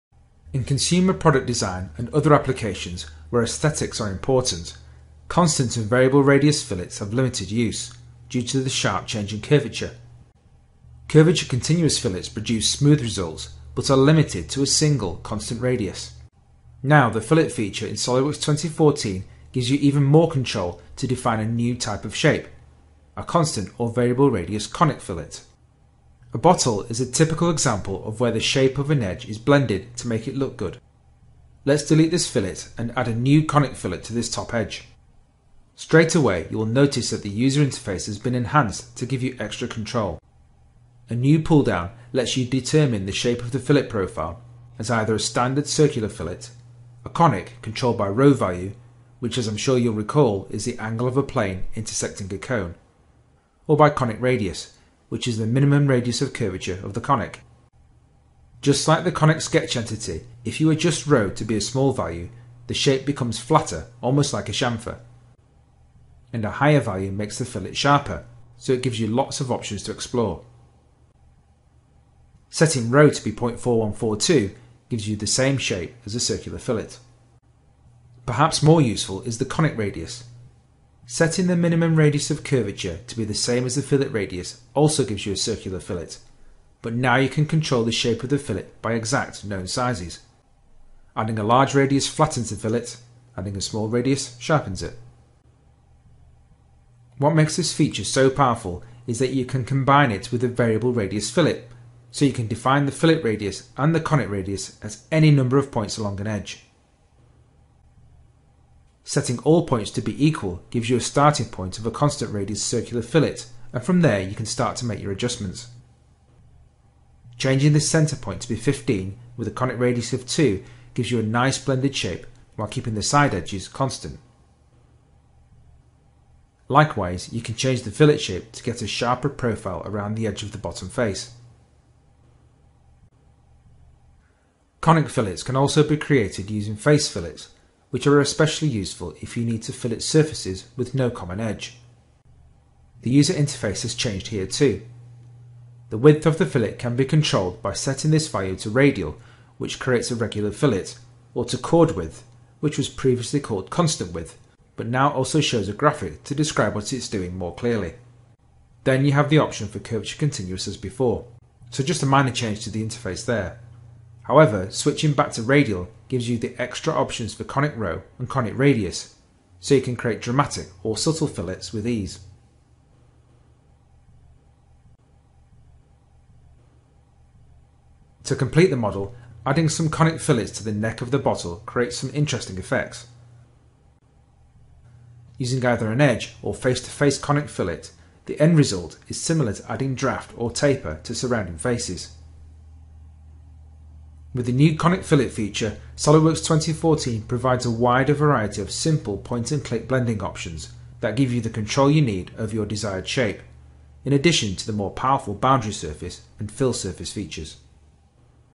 SolidWorks 2014 What's New - Conic Fillets
There's an entirely new kind of fillet in SolidWorks 2014. See how it works.